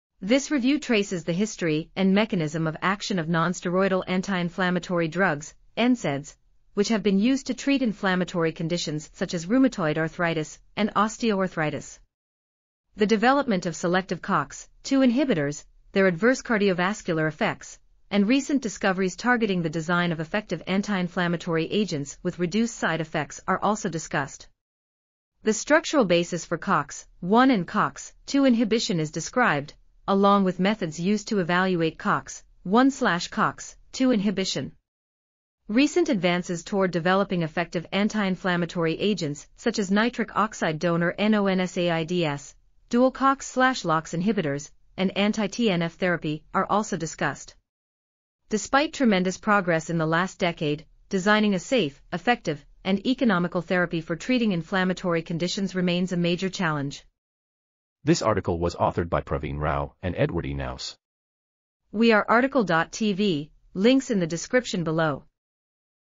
Evolution of Nonsteroidal Anti-Inflammatory Drugs (NSAIDs): Cyclooxygenase (COX) Inhi... | RTCL.TV
### Keywords ###

### Article Attribution ###
Title: Evolution of Nonsteroidal Anti-Inflammatory Drugs (NSAIDs): Cyclooxygenase (COX) Inhibition and Beyond
Authors: Praveen Rao ,and Edward E. Knaus
Publisher: Canadian Society for Pharmaceutical Sciences
DOI: 10.18433/J3T886

### Video Timestamps ###
0:00:00 - Summary
0:00:57 - Title
0:01:01 - Tail
0:01:06 - End